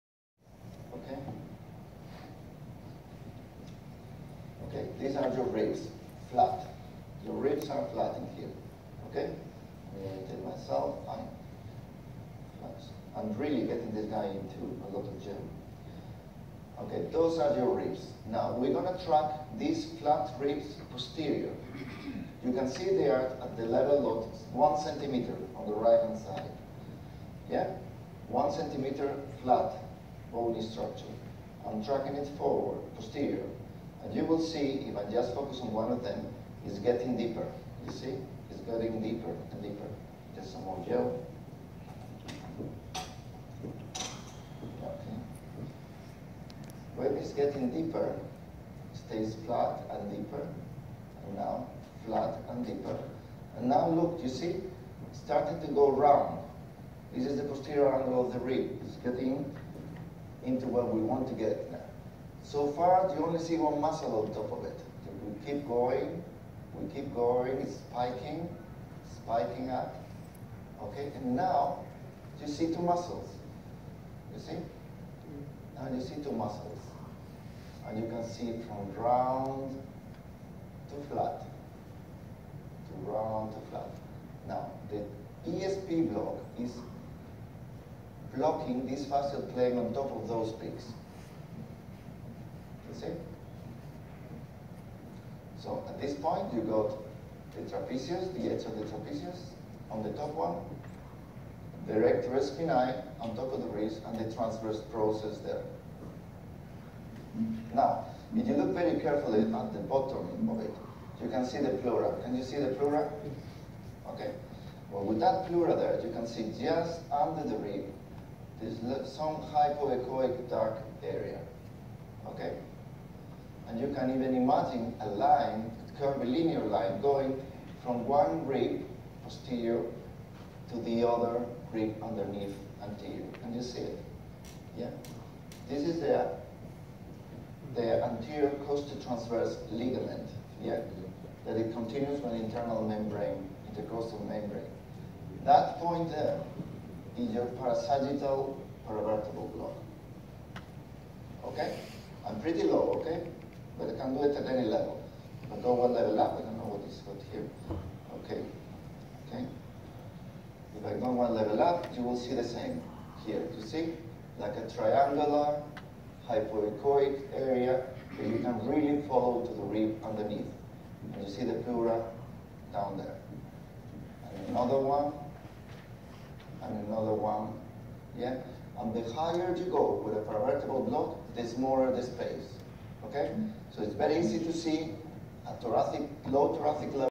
NYSORA Dubai 2019 USG Erector Spinae Plane (ESP) Block
NYSORA Dubai 2019 ultrasound guided Erector Spinae Plane block by Rafael Blanco